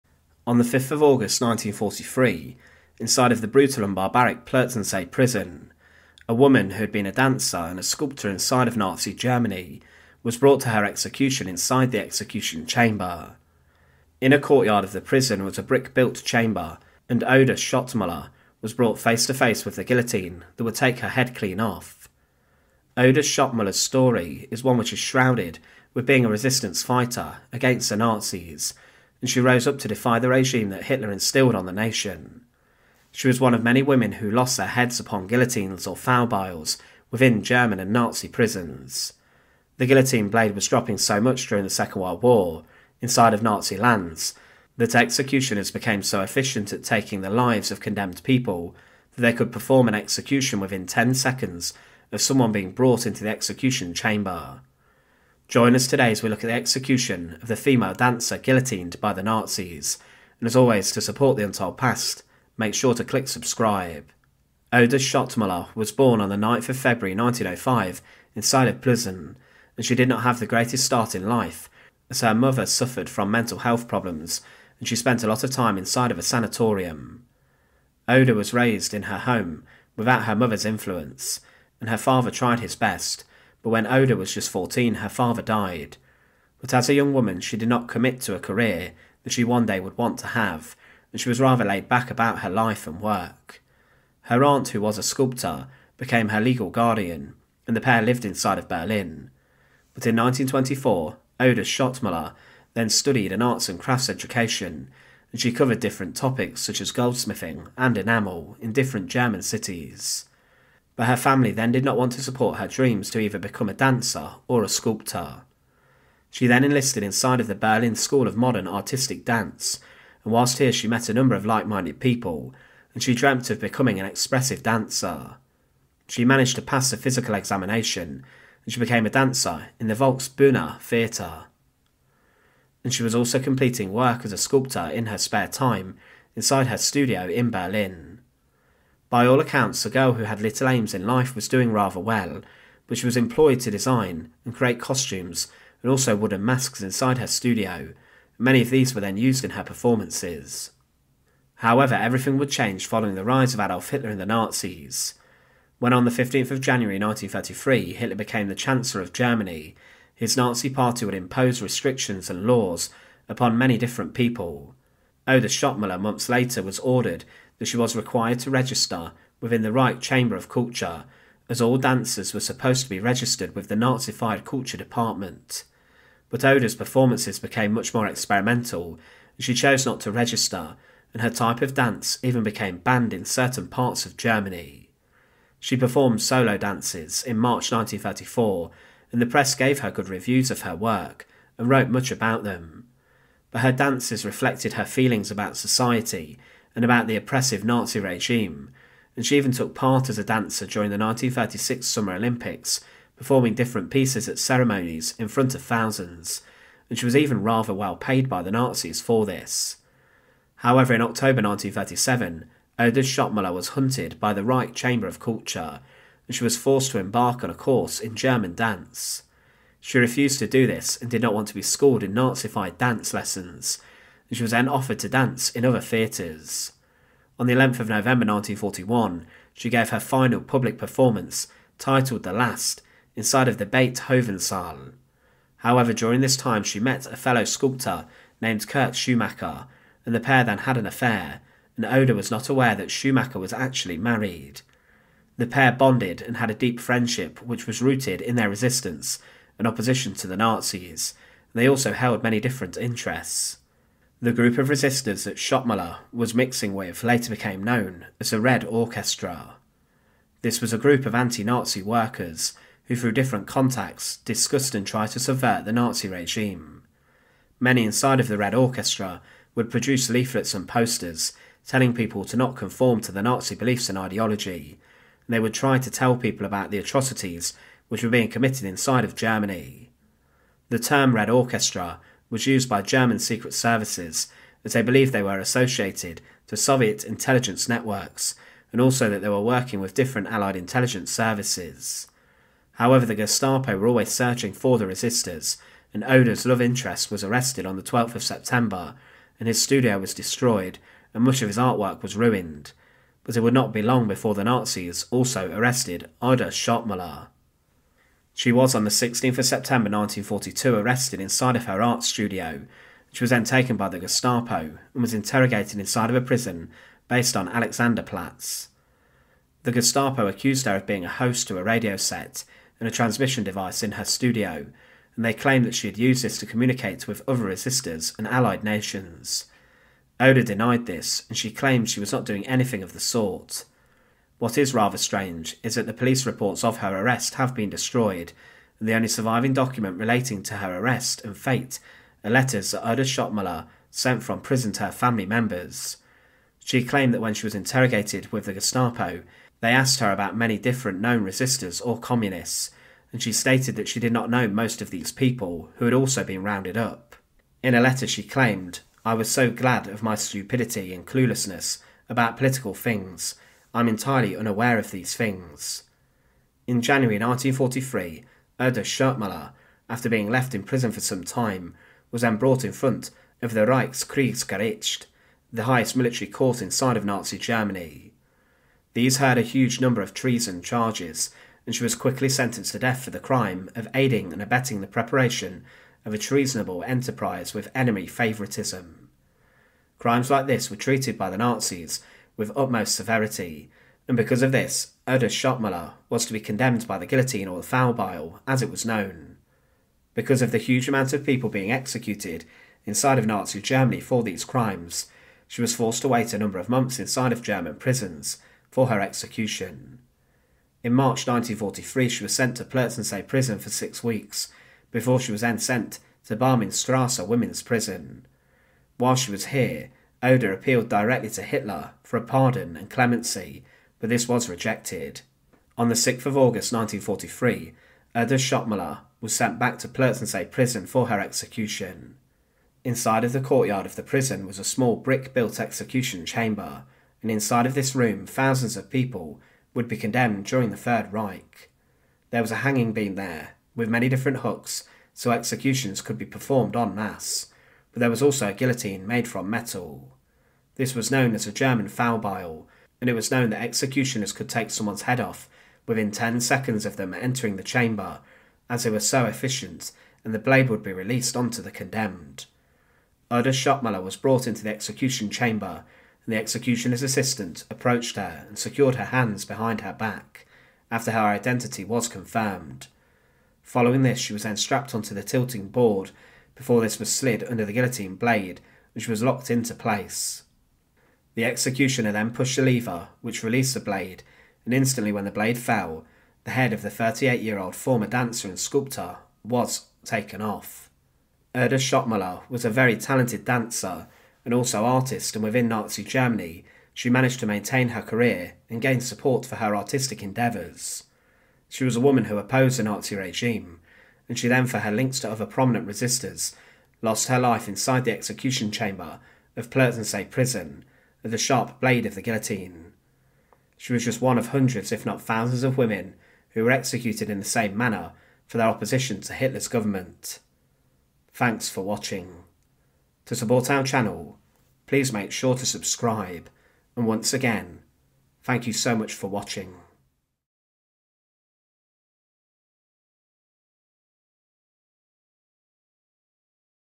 This Female Dancer Was Guillotined For Standing Up To Hitler
Throughout the Second World War, there were many people who rose up and resisted the Nazis and the German war effort. But some German women who displayed resistance were then executed by guillotine inside of one of the darkest Nazi prisons, Plotzensee Prison. Oda Schottmuller was a young dancer who was also a sculptor and she gained favourable reviews across Germany, however she then was implicated in being a resistor.

Because of this she was taken to court and she was then sentenced to death having been found guilty of subversion. The Nazis thought she was a spy, however inside of Plotzensee Prison's execution chamber she was executed on the metal guillotine known as the Fallbeil.

Join us today as we look at, 'The HORRIFIC Execution Of The Female Dancer Guillotined By Hitler'